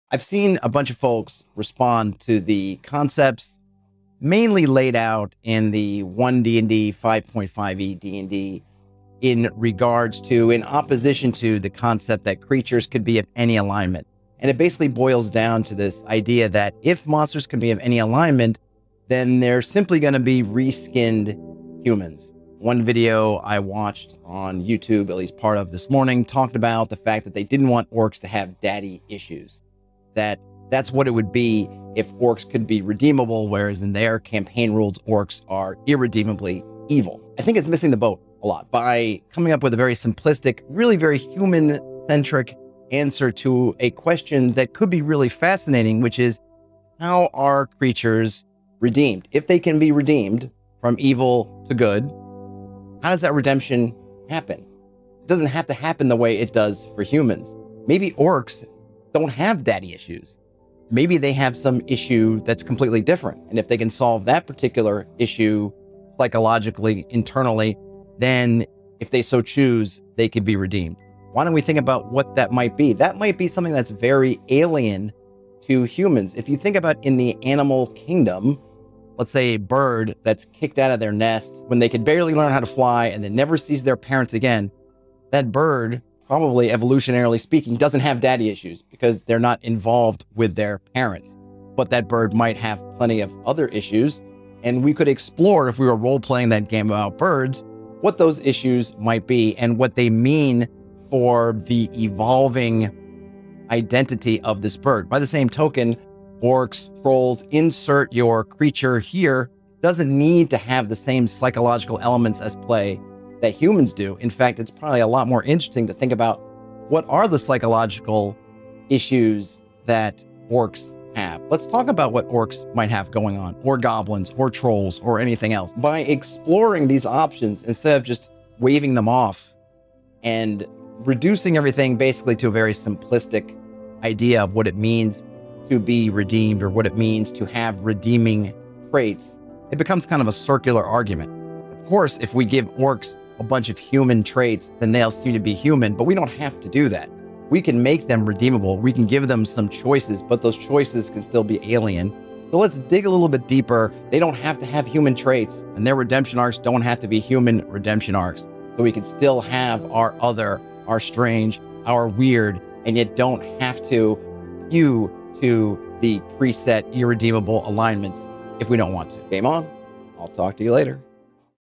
Monsters, Alignments, and the Possibilities for Redemption in D&D (Do Orcs Have Daddy Issues?)
Is there a path through which we can decouple creatures from fixed alignments without turning them into simple re-skinned humans in D&D style fantasy roleplaying games? I have a thought.

Have something you'd like me to preview, review, or otherwise cover on the channel?